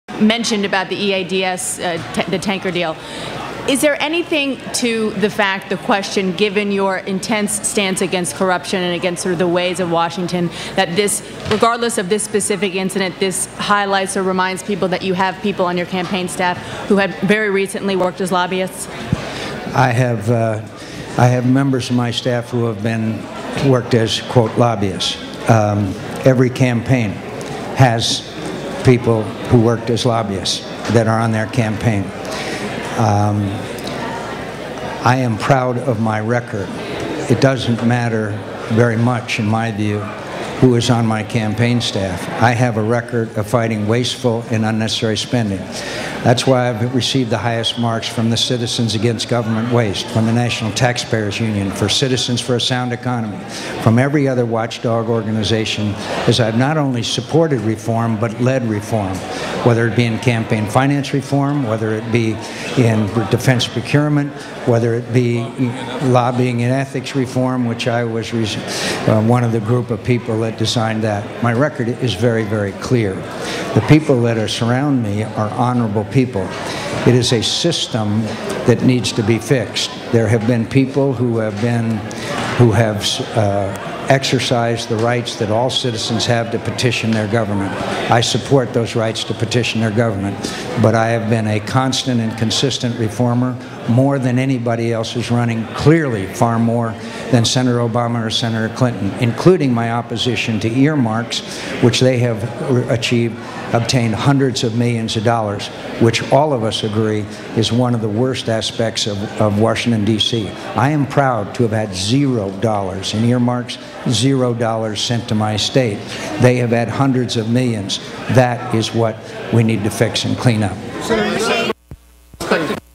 John McCain: Town Hall 03/12/08
John McCain
Town Hall
Exeter, NH 03/12/08

Transcript:
Dana Bash: "Is there anything to the fact of question given your intense stance against corruption and against the ways of Washington that this, regardless of this specific incident, this highlights or reminds people that you have people on your campaign staff who have very recently worked as lobbyists?"

McCain: "I have members of my staff who have been, who have worked as quote lobbyists. Um, every campaign has people who worked as lobbyists on their campaign. Uh, I am proud of my record, it doesn't matter very much, in my view, who is on my campaign staff. I have a record of fighting wasteful and unnecessary spending...

"I have not only supported reform, but led reform.

"My record is very very clear. The people that surround me are honorable people. It is a system that needs to be fixed. There have been people who have been, who have exercised the rights that all citizens have to petition their government. I support those rights to petition their government. But I have been a constant and consistent reformer, more than anybody else is running. Clearly, far more than Senator Obama or Senator Clinton. Including my opposition to earmarks, which they have achieved, obtained hundreds of millions of dollars, which all of us agree is one of the worst aspects of Washington D.C. I am proud to have zero dollars of earmarks, zero dollars sent to my state. They have had hundreds of millions. That is what we need to fix and cleanup."

Label: JM NH 3-12 (JC#118) CDGHK - ClipQ